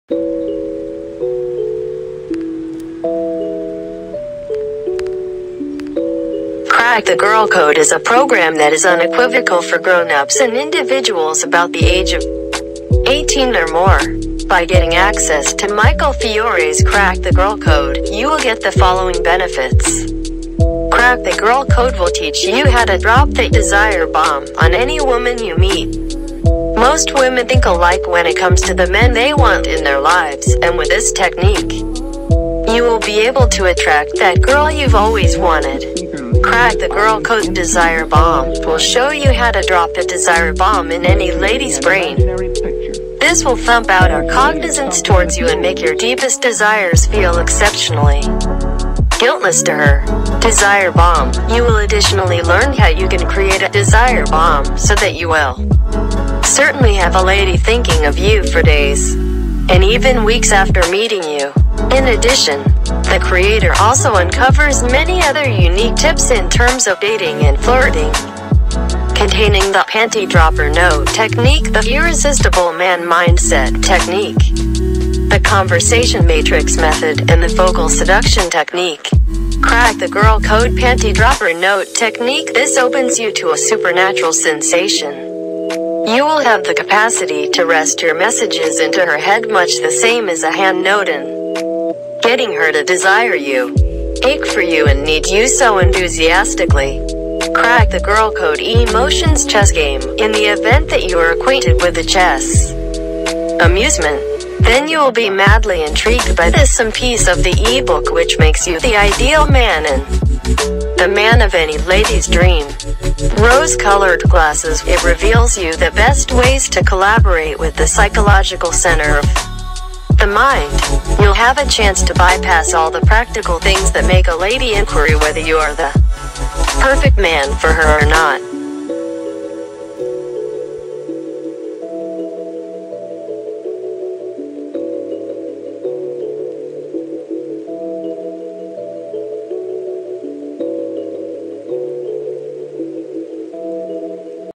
Crack The Girl Code Review - Is it Worth Buying? Must Read This!
Crack The Girl Code is a program that is unequivocal for grown-ups and individuals about the age of 18 or more. By getting access to Michael Fiore’s Crack the Girl Code, you will get the following benefits. Crack the Girl Code will teach you how to drop the “Desire Bomb” on any woman you meet. Most women think alike when it comes to the men they want in their lives, and with this technique, you will be able to attract that girl you’ve always wanted. Crack The Girl Code Desire Bomb – will show you how to Drop a Desire Bomb in any lady’s brain, this will thump out are cognizance towards you and make your deepest desires feel exceptionally guiltless to her。 Desire Bomb – You will additionally learn how you can create a “Desire Bomb” so that you will certainly have a lady thinking of you for days, and even weeks after meeting you.

In addition, the creator also uncovers many other unique tips in terms of dating and flirting, containing the “panty dropper note” technique, the “irresistible man mindset” technique, the conversation matrix method, and the vocal seduction technique. Crack The Girl Code Panty Dropper Note Technique – This opens you to a supernatural sensation, you will have the capacity to rest your messages into her head much the same as a hand note and getting her to desire you, ache for you and need you so enthusiastically。 Crack The Girl Code Emotions Chess Game – in the event that you are acquainted with the chess amusement, then you will be madly intrigued by this some piece of the eBook which makes you the ideal man and the man of any lady’s dream. Rose Colored Glasses – It reveals you the best ways to collaborate with the psychological center of the mind. You’ll have a chance to bypass all the practical things that make a lady inquiry whether you are the perfect man for her or not.

The vocal technique will make you understand the power that is in your voice and how to use it to turn her on. This voice technique has rhythm and emotion that you can combine to pass your point into her mind deeply. All of these techniques have been tested and proven effective several times, so you will surely get a lot of panties dropping wherever you go. It will teach you all the captivating, catchy words and phrases that you can use to help you make that lady to fall for you instantly. All these catchy and captivating words and phrases will be explained to you on where and how to use them. In addition to all the information in the Crack the Girl Code, Michael Fiore is also giving guys several advanced trainings for free to help them attract women even more effectively with simple steps and tricks, says Michaels. “Thousands of guys have already found that with this guide, getting with women is easy—they’re easy to talk to, easy to understand, and easy to date.”

The program contains guides, audios and video tutorials on how to get the woman of your dreams want you not just for a short time but in a long- term basis. It includes three main modules namely: Self- Awareness and Self- Assurance, Understanding Women and The Steps and Strategies To Get The Women You Want. When ordering Michael Fiore’s program, you also get a 60-day Money Back Guarantee, which means you have up to 60 days to try out this program with NO RISK.